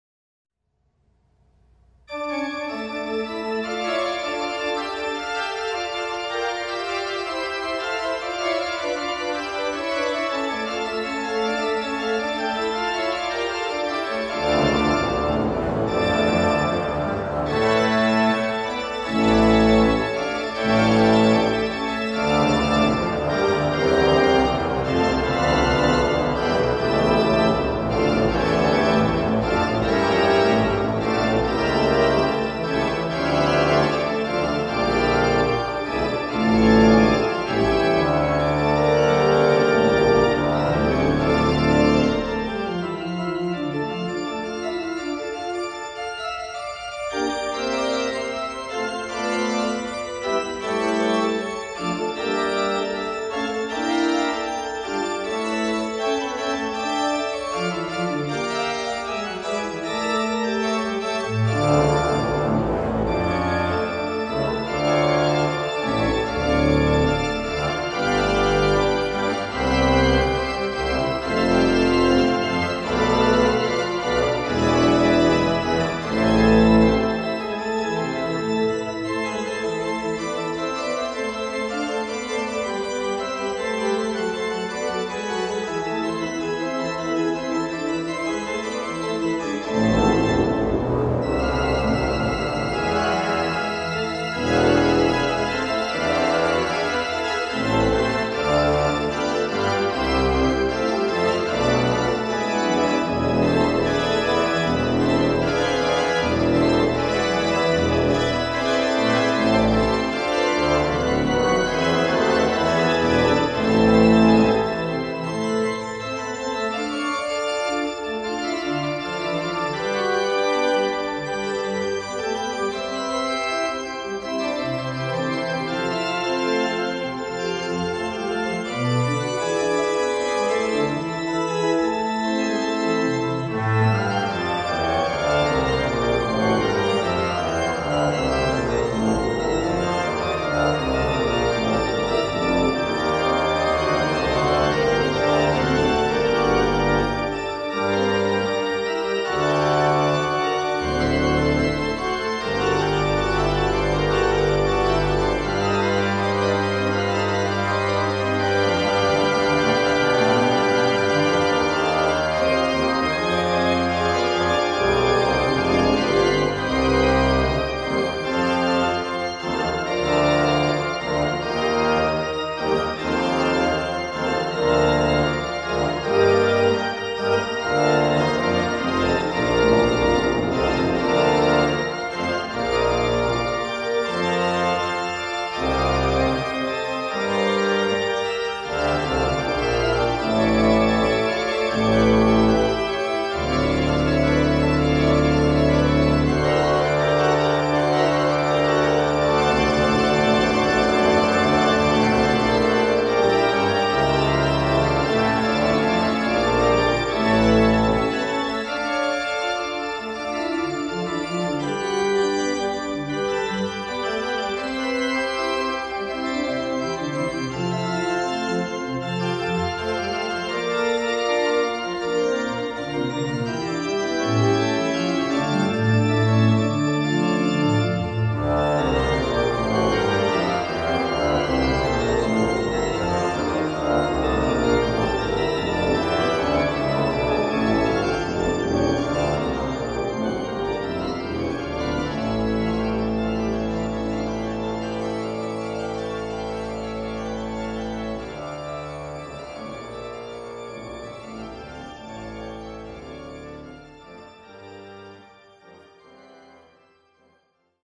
52slideB.avi
Construction photo slide-show of Jaeckel Organ's Opus 52 pipe organ in Duluth, Minnesota.